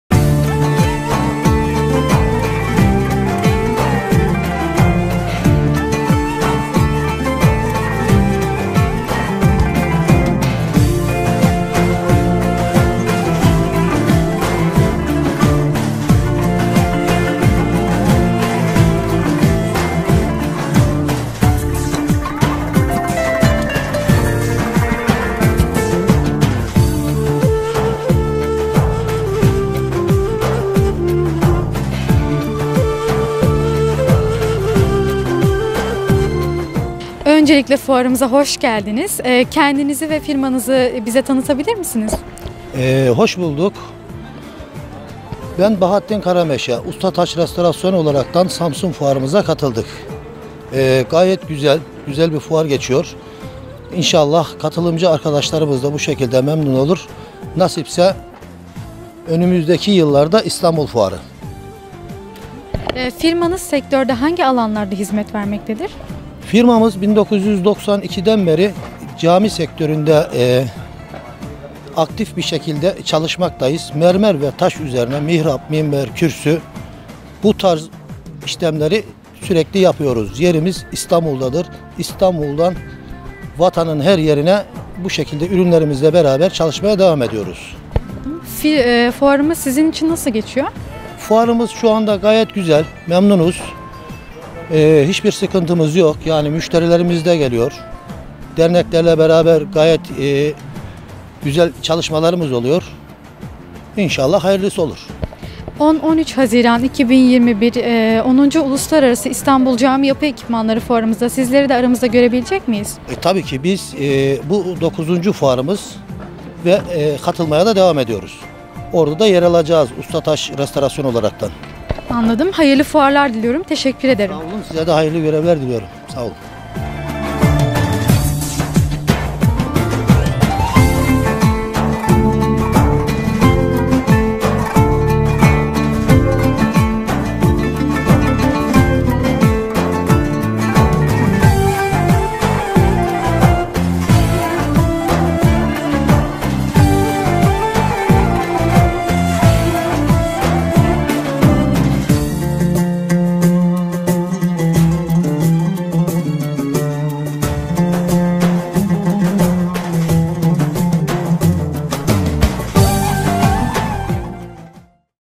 Bahattin KARAMEŞE | Usta Taş Restorasyon Ltd. Şti. | 9. Cami Yapı Ekipmanları Fuarı | SAMSUN
10-13 Eylül 2020 9. Uluslararası Cami Yapı Ekipmanları Fuarımıza katılmış olan Usta Taş Restorasyon Ltd. Şti. firmasının yetkilisi Bahattin KARAMEŞE Beyin röportajı...